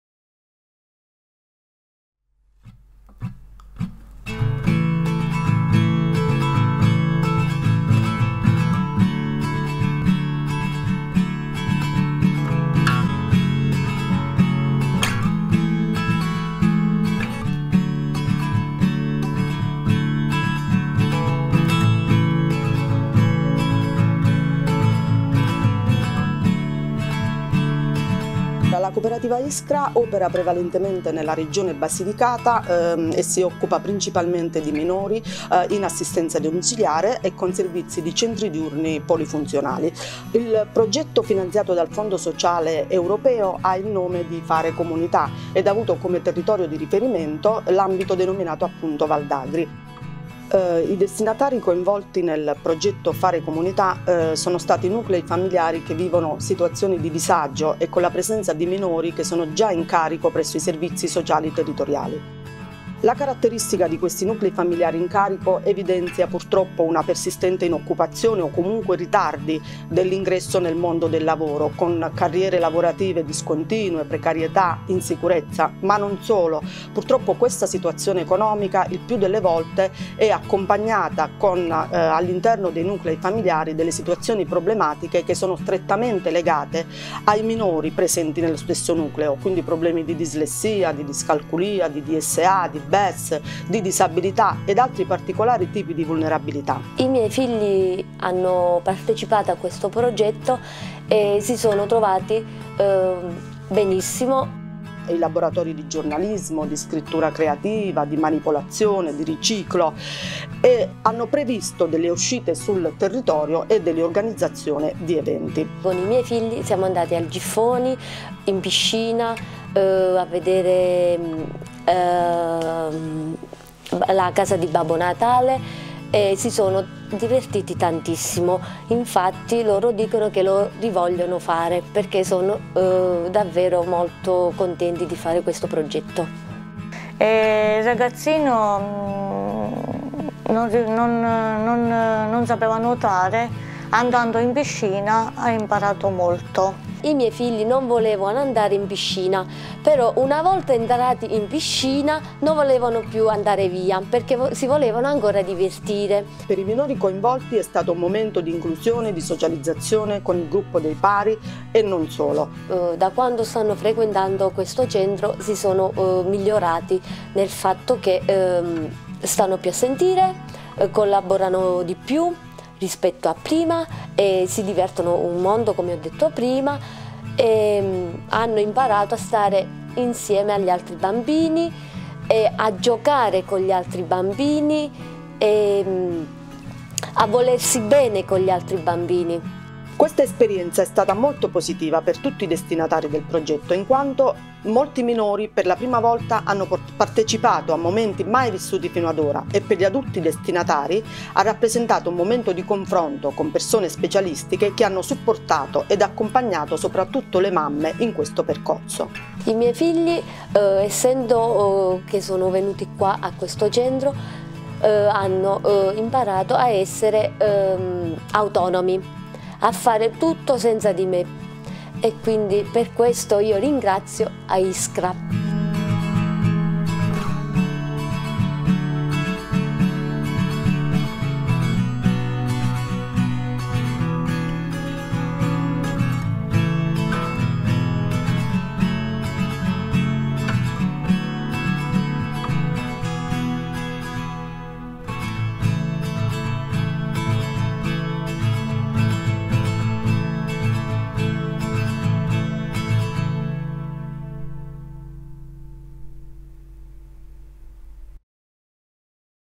Alla scoperta del mondo: cultura, sport e arte nei laboratori per minori in difficoltà (vers. corta)
Episodio 4 - Il laboratorio della felicità - Racconti ed esperienze dei progetti di accompagnamento per persone in condizioni di vulnerabilità e fragilità sociale

Progetto "Fare Comunità" di Cooperativa sociale Iskra, Associazione Tolbà, Cooperativa sociale Social servizi